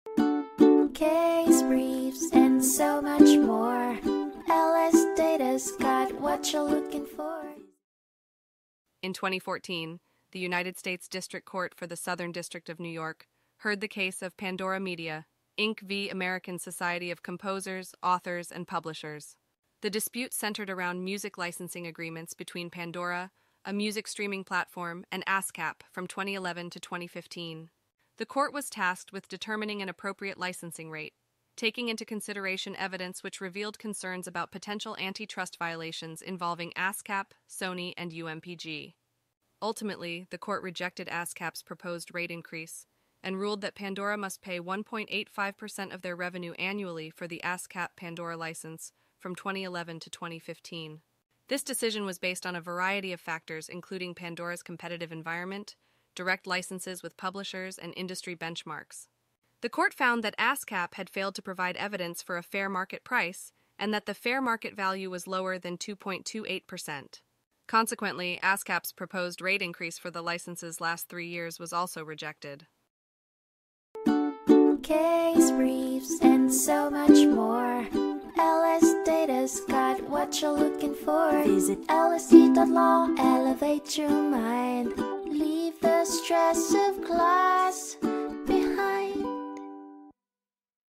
In re Pandora Media, Inc. v. American Society of Composers, Authors, & Publishers (2014) Overview
Pandora and ASCAP had a dispute over music licensing agreements from 2011 to 2015. Evidence revealed concerning coordination between ASCAP, Sony, and UMPG, raising antitrust concerns. The court rejected ASCAP's proposed rate increase and concluded that Pandora must pay 1.85% of their revenue each year for the ASCAP-Pandora license from 2011 to 2015.

In re Pandora Media, Inc. v. American Society of Composers, Authors, & Publishers (2014)
United States District Court for the Southern District of New York
6 F. Supp. 3d 317